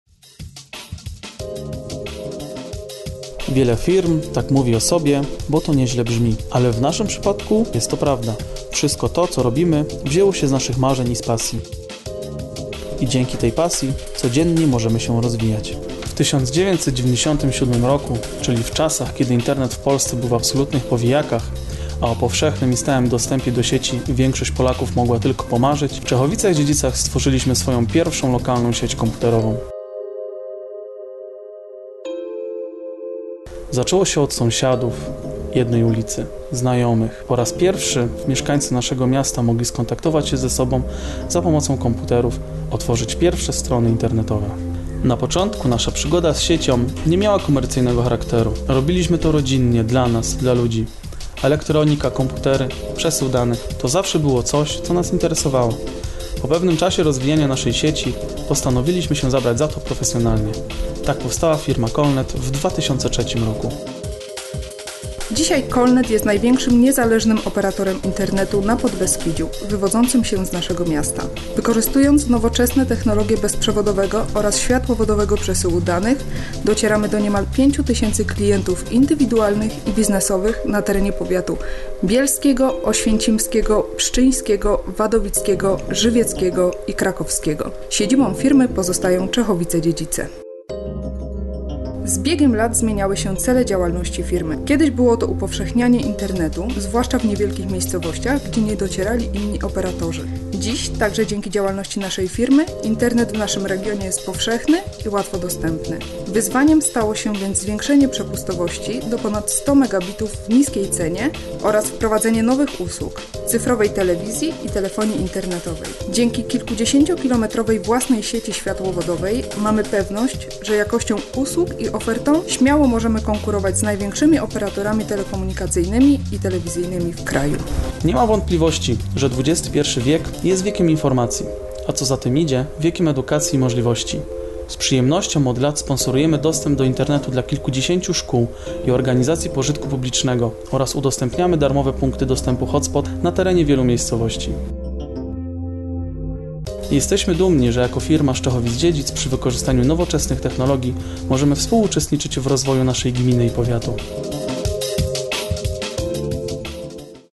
KOLNET - Film promocyjny - Czechosław 2012
Film prezentowany na gali "Czechosław 2012" w Czechowicach-Dziedzicach